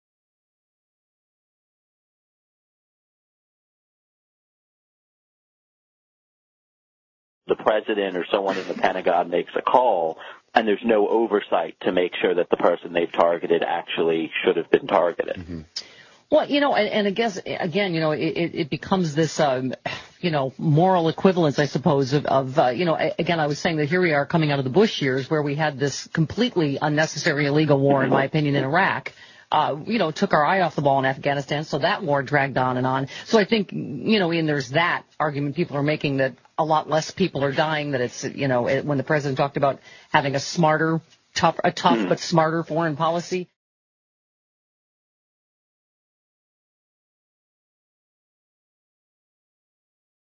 Libs Defend Obama's Drone Kills: 'Smarter & Tougher' Wars
IAN MILHISER (06 Feb 2013): The President or someone in the Pentagon makes a call and there's no oversight to make sure that the person they've targeted actually should have been targeted.

STEPHANIE MILLER: Well you know an an an and I guess again you know it it it becomes this um you know moral equivalence I suppose of a you know again I was saying here we are coming out of the Bush years where we had this completely unnecessary illegal war in my opinion in Iraq.

Ah you know took our eye off the ball in Afghanistan so that war dragged on and on. So I think you know Ian there's that argument people are making that a lot less people are dying that it's when the President talked about having a smarter tough a tough, but smarter foreign policy.